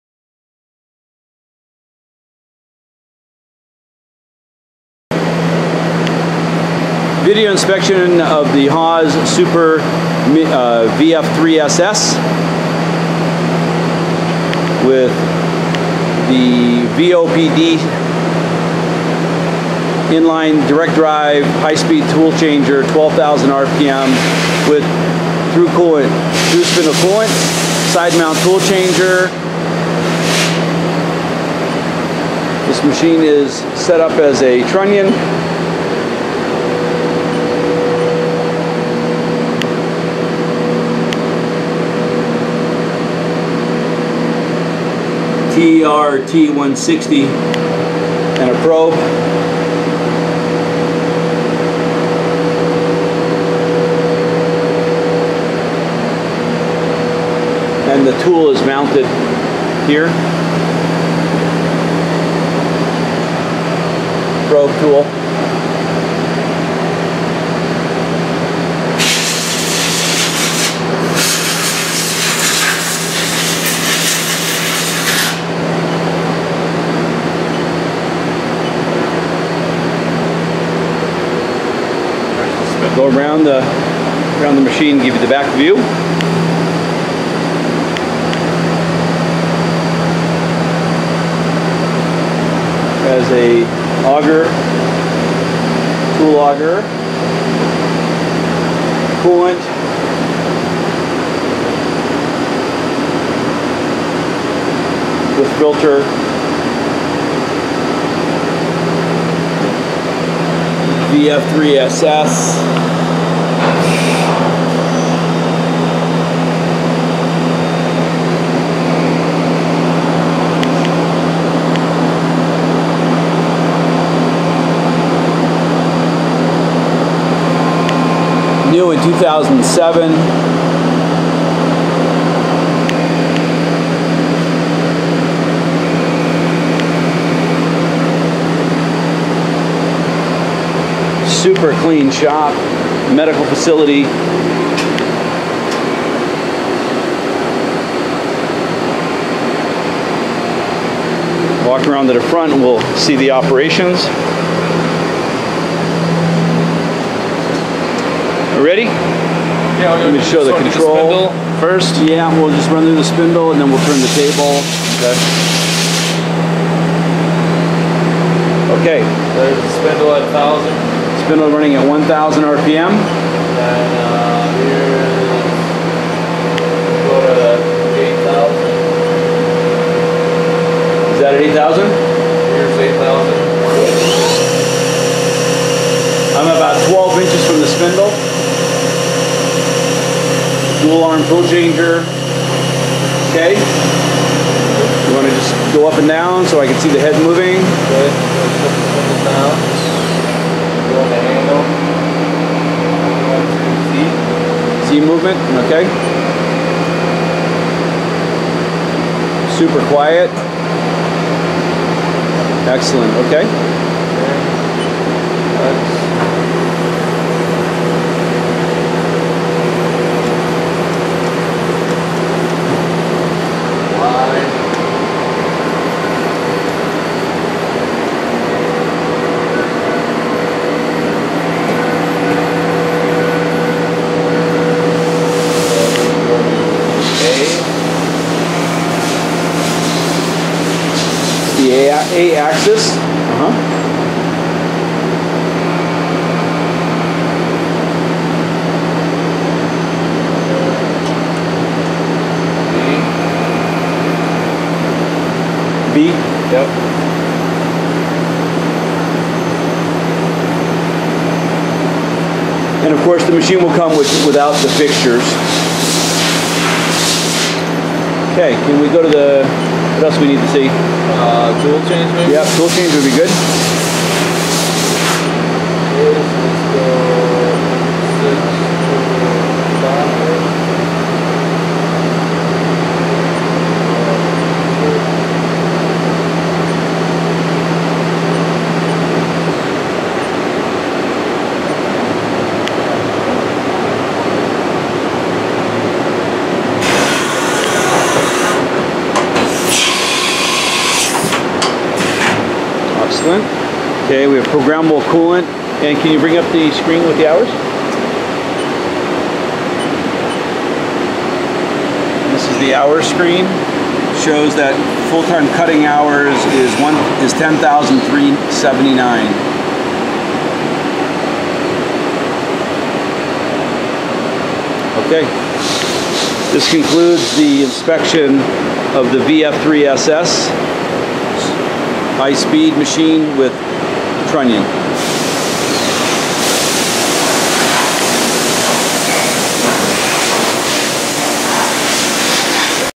Haas VF-3SS Vertical Machining Center m/c 403810
"To the best of our knowledge all details listed below are deemed correct.
It is the buyer's responsibility to confirm prior to purchase."

Equipped With:
   SIDE MOUNT TOOL CHANGER WITH HIGH SPEED TOOL CHANGE
   PROGRAMMABLE COOLANT NOZZLE
   TSC - THROUGH SPINDLE COOLANT
   12,000 RPM SPINDLE
   PULSE REMOTE HANDLE
   HAAS TR160 TRUNNION
   RENISHAW OMP 40-2 PROBE
   INLINE DIRECT DRIVE SPINDLE

Specifications:
   X-AXIS TRAVEL (TABLE):                           40"
   Y-AXIS TRAVEL (SADDLE):                          20"
   Z-AXIS TRAVEL (SPINDLE HEAD):                    25"
   SPINDLE END TO TABLE SURFACE:                    4" TO 29"
   TABLE SIZE:                                      48" X 18" TABLE LOAD
   TAPER OF SPINDLE HOLE:                           NO. 40
   SPINDLE SPEED RANGE:                             12,000 RPM
   RAPID TRAVERSE SPEED (X/Y/Z):                    1000 IPM
   CUTTING FEEDRATE (X/Y/Z):                        200 IPM
   MAIN SPINDLE DRIVE MOTOR:                        20 HP
   MAGAZINE TOOL CAPACITY:                          24
   FLOOR SPACE REQUIRED (APPROX.):                  74.4" X 110" X 119" H

UNIT IS COMING OUT OF NON-PRODUCTION MEDICAL FACILITY
CONDITION: EXCELLENT TO MINT